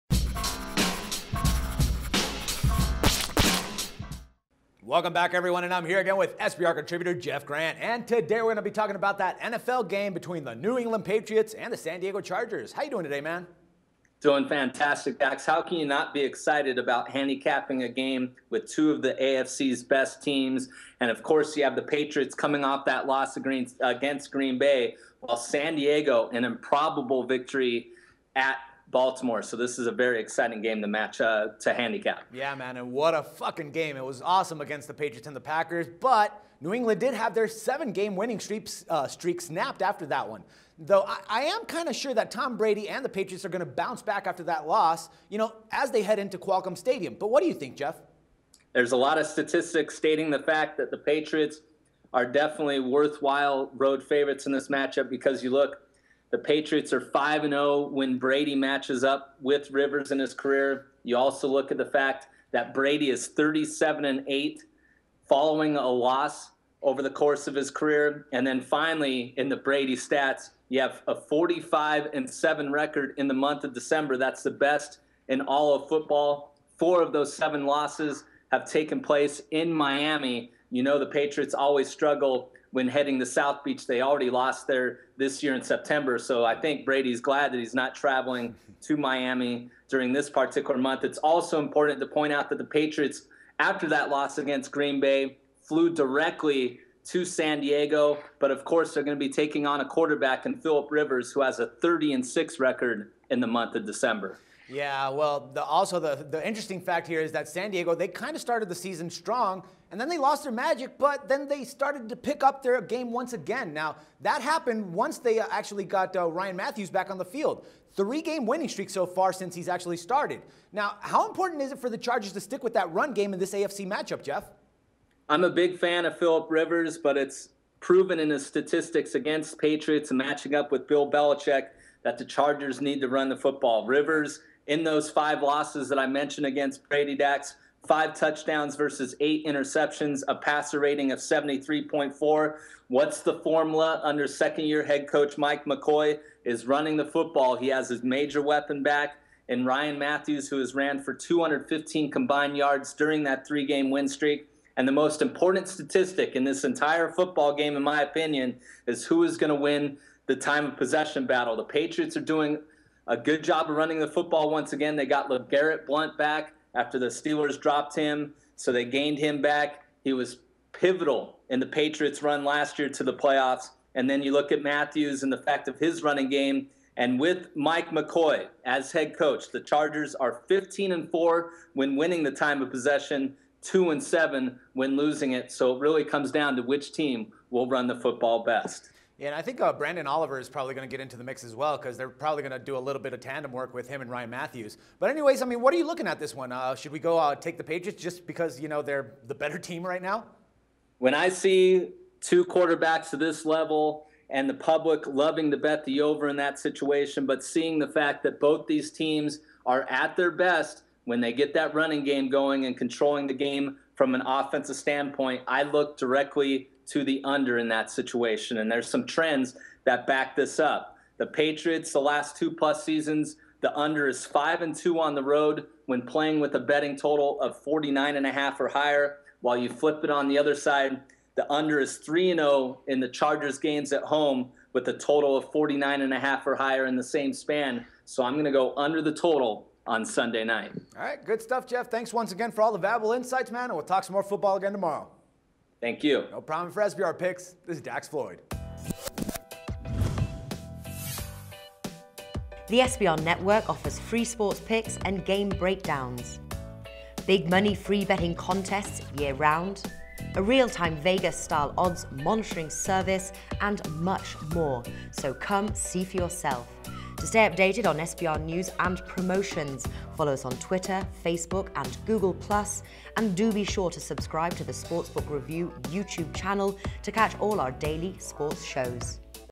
NFL Picks for Sunday Night Football: Patriots vs. Chargers
A huge game between the Patriots and the Chargers could be the deciding factor of who will take the No. 1 seat in the AFC. Have an edge on the NFL odds in the game with Sportsbook Review's handicapper Jeff Grant who gave his free NFL picks on the matchup.